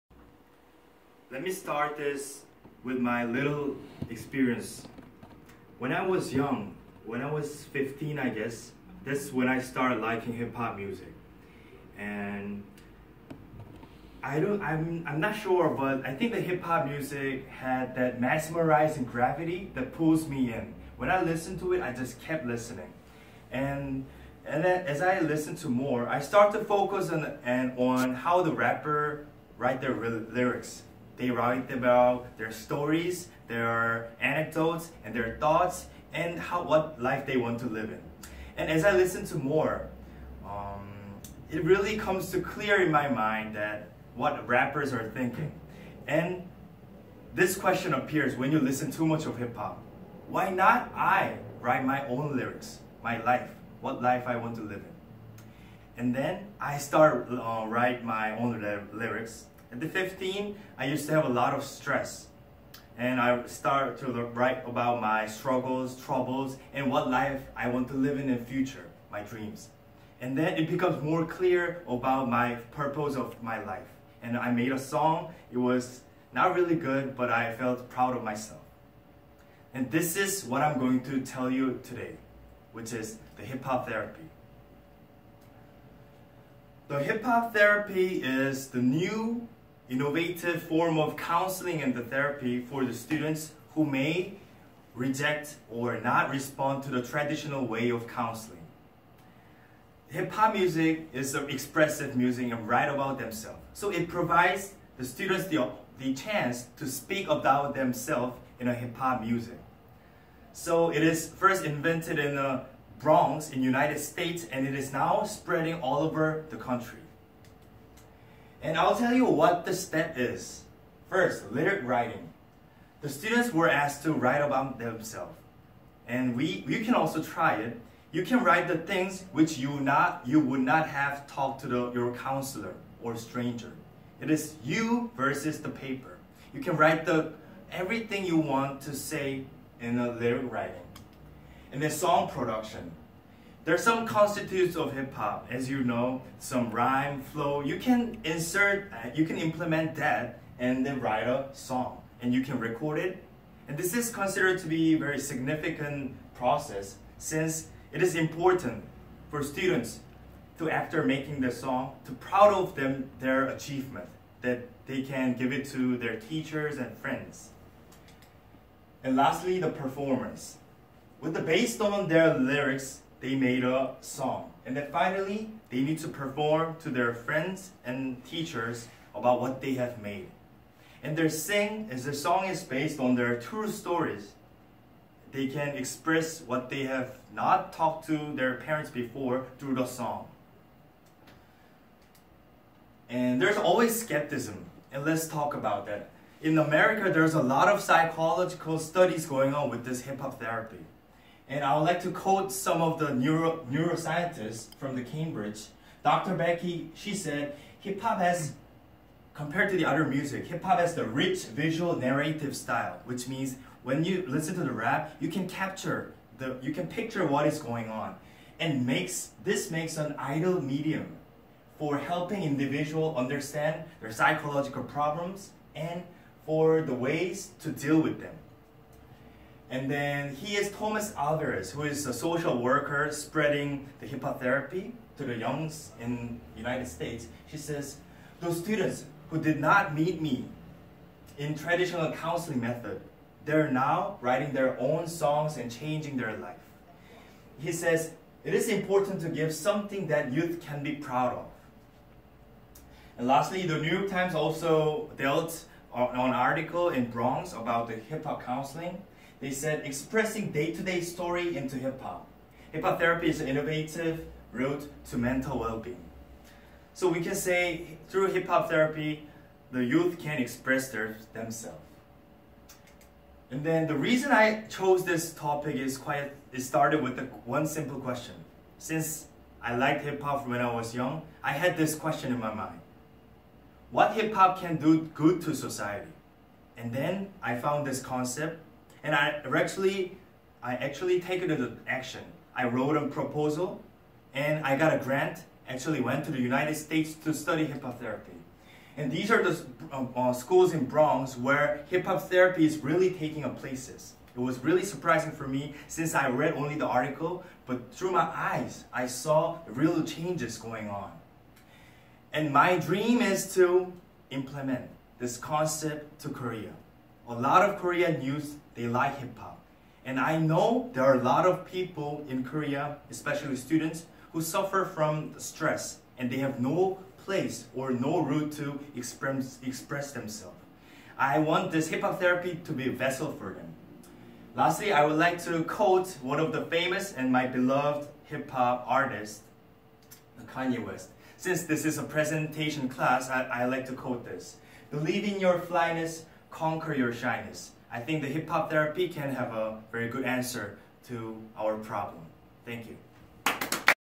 Hoon Young Heo / Get to the Point(1st Assignment) / Wed 1-3pm
Presentation about Hip-Hop therapy